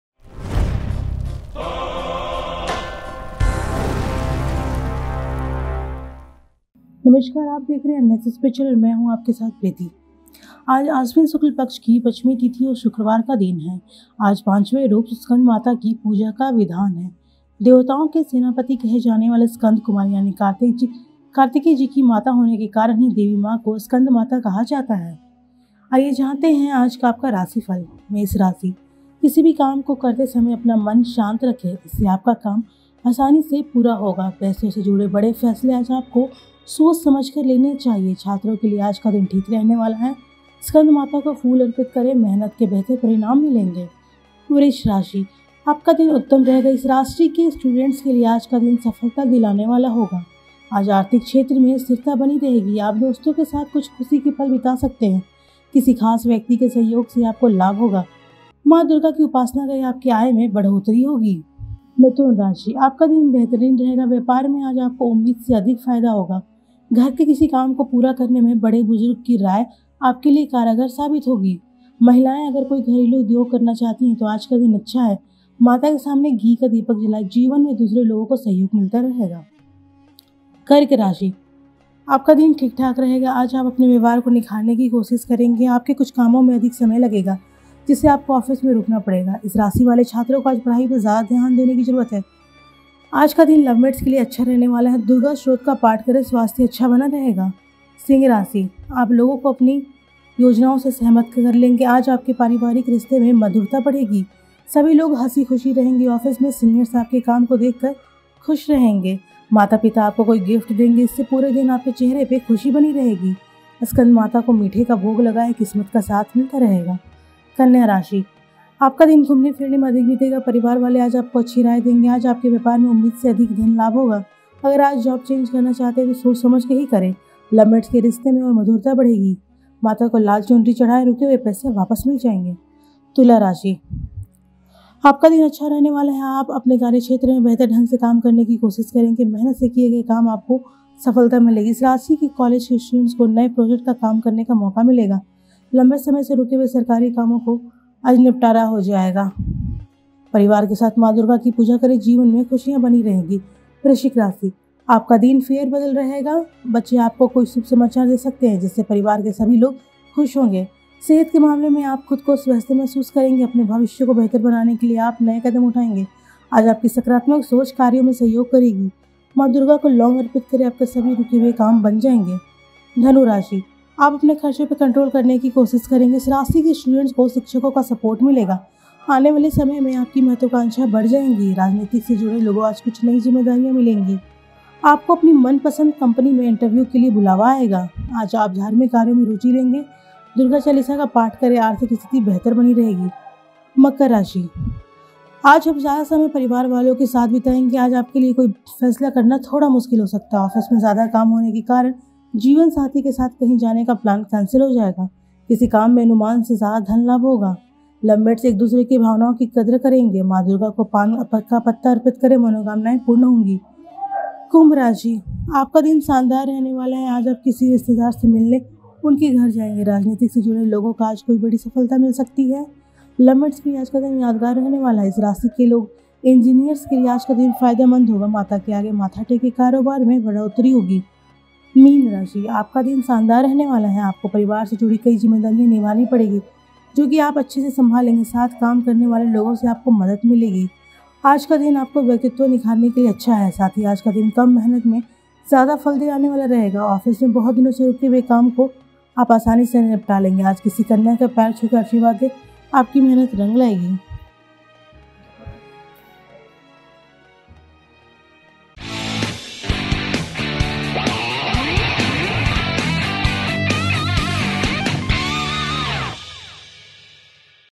Rashifal : नवरात्रि के पांचवें दिन मां स्कंदमाता हरेंगी आपके सारे दुख, इन 4 राशियों पर बरसाएंगी कृपा
aajkarashifal